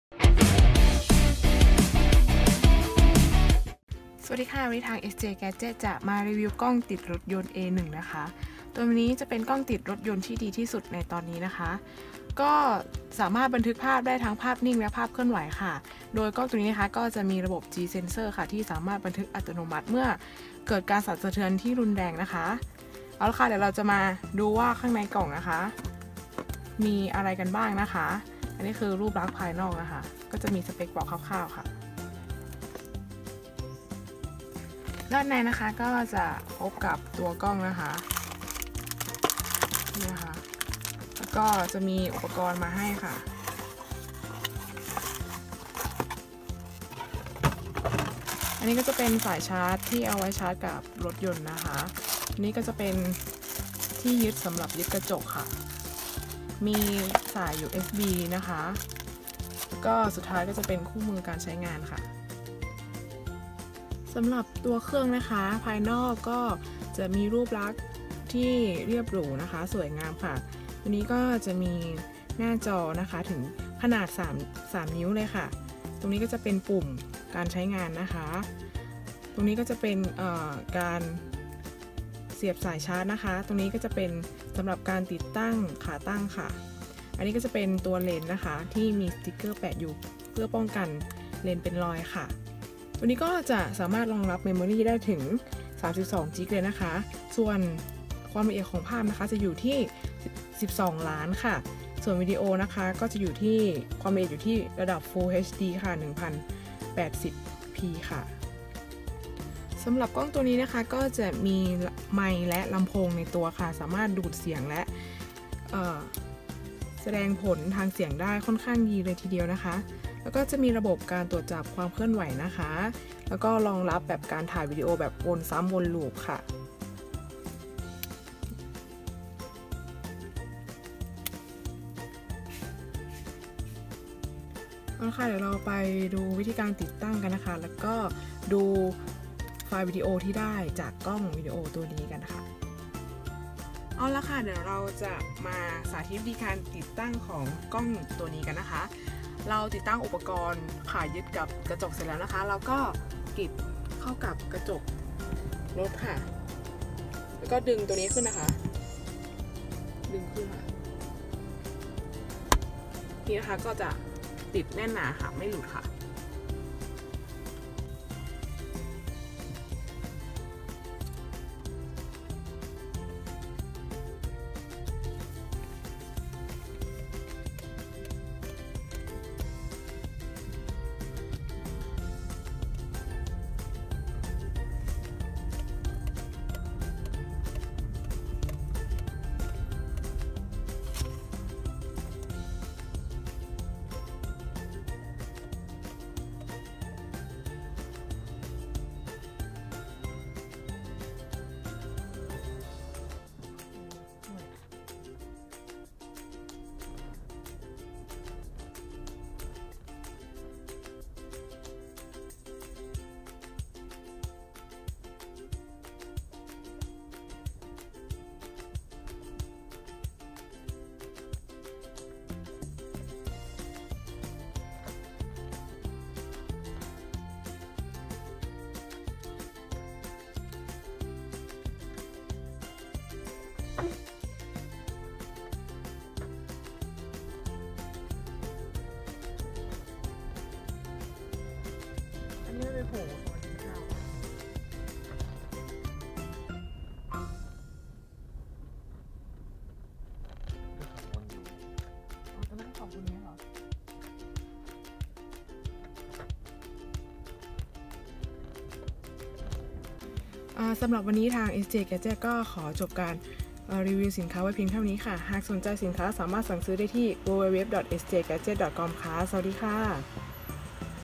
กล้องติดรถ A1 full HD 🚘REVIEW🚘
*** SJgadget ***  เก็บเงินปลายทางทั่วประเทศ

🐶 LINE : @SJgadget  (มีตัว @ด้วย)

🚘 ความละเอียดของวิดีโอที่บันทึก มี 4 ระดับ คือ
    Full HD 1080P (1920x1080) 30FPS
    720P (1280x720) 30FPS
    WVGA (848x480)
    VGA (640x480)
- ความละเอียดของรูปภาพ มี 8 ระดับ คือ
    12M (4032x3024)
    10M (3648x2736)
    8M (3264x2448)
    5M (2592x1944)
    3M (2048x1536)
    2MHD (1920x1080)
    1.3M (1280x960)
    VGA (640x480)
🚘 มุมมองกล้องกว้าง 170 องศา ครอบคลุมพื้นที่หน้ารถได้ทุกรุ่น
- รูรับแสงขนาด F2.2 ทำให้ถ่ายภาพตอนกลางคืนชัดยิ่งขึ้น
- ขนาดหน้าจอ 3.0 นิ้ว ดูได้สบาย ไม่ปวดตา
🚘 ระบบ WDR อัตโนมัติ เพิ่มความคมชัดของภาพในเวลากลางคืน
- รองรับการนำภาพขึ้นหน้าจอทีวีผ่านช่อง HDMI
- มีระบบ G-Sensor ระบบบันทึกอัตโนมัติเมื่อกล้องเกิดการสั่นสะเทือน หรือกระแทก
🚘 รองรับ Micro SD card สูงสุด 32 GB
- มีไมค์และลำโพงในตัว
- สามารถย้อนดูภาพหรือวีดีโอภายหลังจากตัวเครื่องได้
🚘 มีระบบตรวจจับความเคลื่อนไหว
- รองรับการถ่ายวีดีโอแบบวนซ้ำ